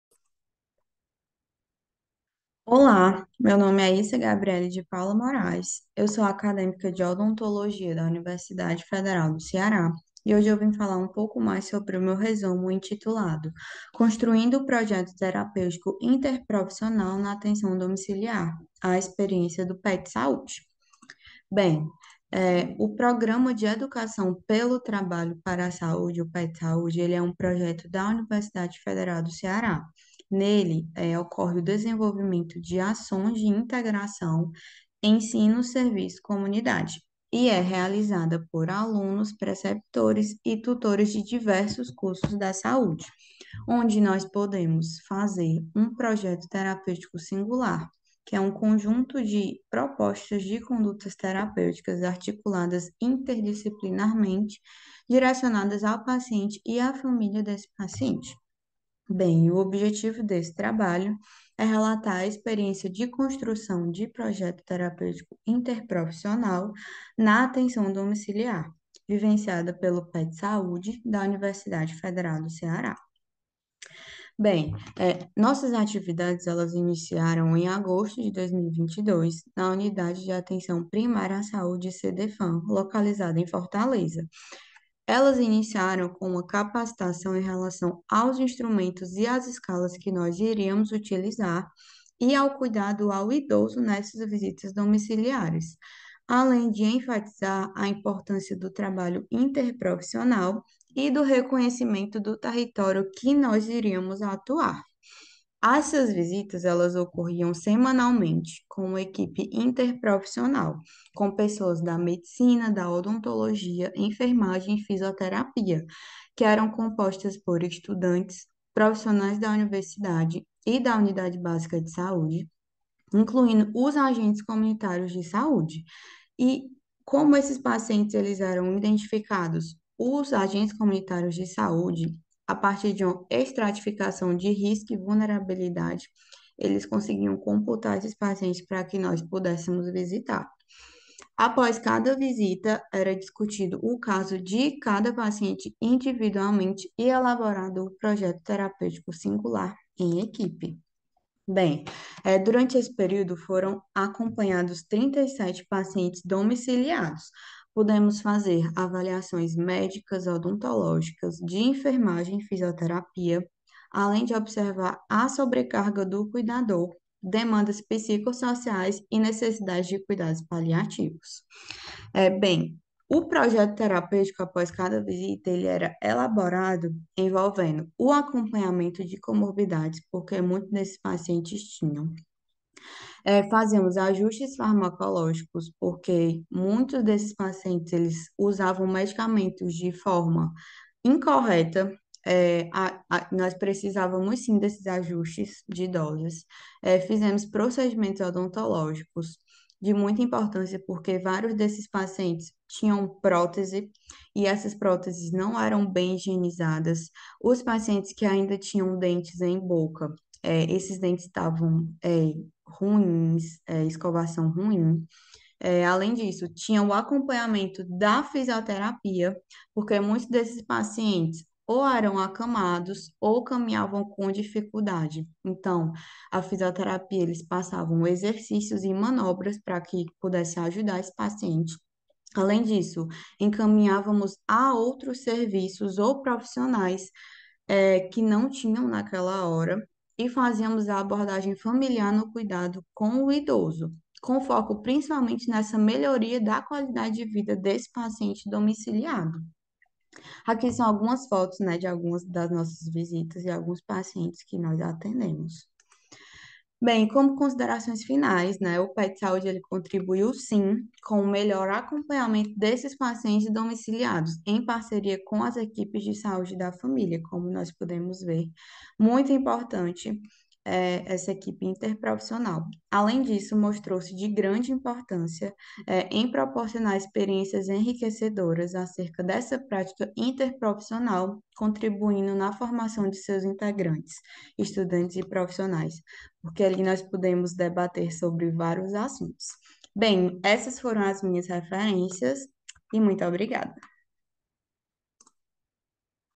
315 - Construindo projeto terapêutico interprofissional na atenção domiciliar A experiência do PET
Autores: Hissa Gabrielle de Paula Morais¹, Ana Karine Macedo Teixeira¹, Isaac Augusto Dantas Nogueira¹, Janaína Saboia Aguiar², Marco Túlio Aguiar Mourão Ribeiro¹.

1 - UFC - Universidade Federal do Ceará , 2 - SMS - Secretaria Municipal de Saúde de Fortaleza

Resumo
Introdução: O Programa de Educação pelo Trabalho para a Saúde (PET-Saúde) permite o desenvolvimento de ações de integração ensino-serviço-comunidade realizadas por alunos, preceptores e tutores de diversos cursos da saúde. Objetivo: Relatar a experiência de construção de projeto terapêutico interprofissional na atenção domiciliar vivenciada pelo PET-saúde da Universidade Federal do Ceará. Metodologia: Trata-se de um relato de experiência de atenção domiciliar no território de abrangência do Posto de Saúde Dr. Gilmário Mourão Teixeira do Centro de Desenvolvimento Familiar (CDFAM – UFC) com uma equipe interprofissional (Medicina, Odontologia, Enfermagem e Fisioterapia) composta por estudantes, profissionais da universidade e profissionais da unidade básica de saúde. As atividades iniciaram em agosto de 2022 com capacitações em relação aos instrumentos e escalas que seriam aplicados e ao cuidado ao idoso nas visitas domiciliares, além de enfatizar a importância do trabalho interprofissional e do reconhecimento do território que seria trabalhado. As visitas domiciliares ocorriam semanalmente e ao final de cada visita era discutido o caso do paciente e elaborado o projeto terapêutico singular em equipe. Resultados: Foram acompanhados 37 pacientes domiciliados que foram identificados a partir de uma estratificação de risco e vulnerabilidade realizada pelos agentes comunitários de saúde. Durante as visitas, realizava-se a avaliação médica, odontológica, de enfermagem e fisioterapia, além de observar a sobrecarga do cuidador, demandas psicossociais e necessidade de cuidados paliativos. O projeto terapêutico era elaborado com foco na melhoria da qualidade de vida do paciente domiciliado, envolvendo o acompanhamento de comorbidades, ajuste farmacológico, procedimentos odontológicos, acompanhamento de fisioterapia, encaminhamento a outros serviços ou profissionais, abordagem familiar nos cuidados com o idoso, proporcionando assim, um cuidado integral ao paciente. Considerações finais: O PET-Saúde permitiu uma troca de experiências de extrema riqueza, não só acadêmica como também pessoal entre os profissionais e estudantes, e que, talvez, não fosse possível apenas com pessoas de um mesmo curso ou segmento. Assim, o projeto contribuiu com um melhor acompanhamento desses pacientes em parceria com as equipes de saúde da família. Mostrando-se de grande importância por proporcionar experiências enriquecedoras acerca da prática domiciliar interprofissional, contribuindo na formação de seus integrantes.